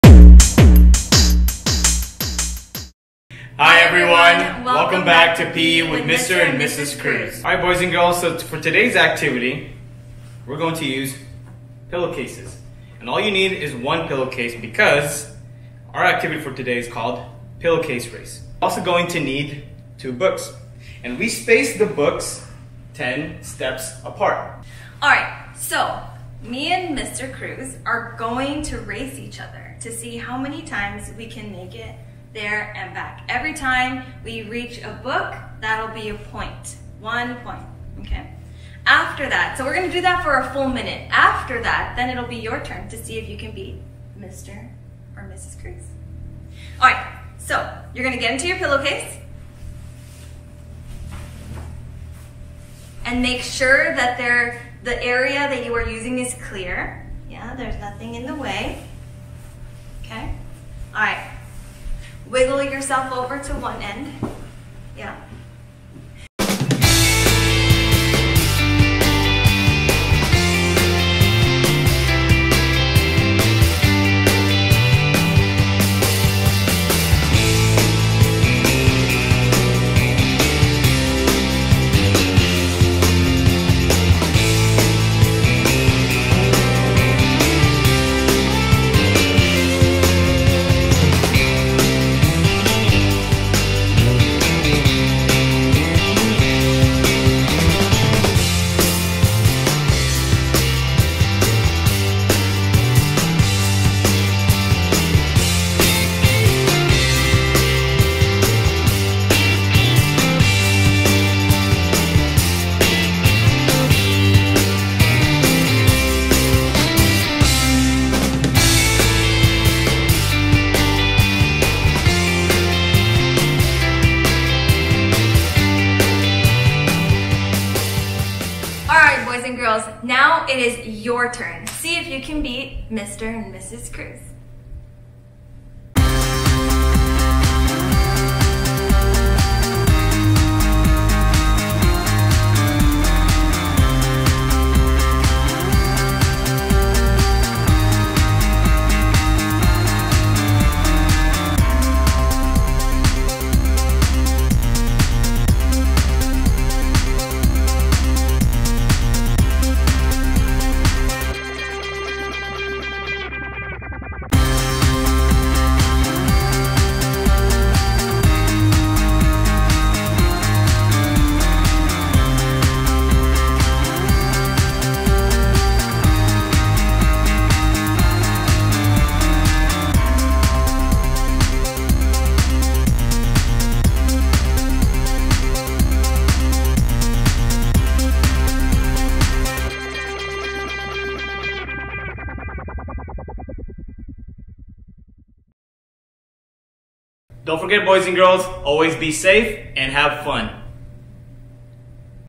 Pillowcase Race
Grab a pillowcase and let's race! Make sure you are safe when completing this activity. Have fun!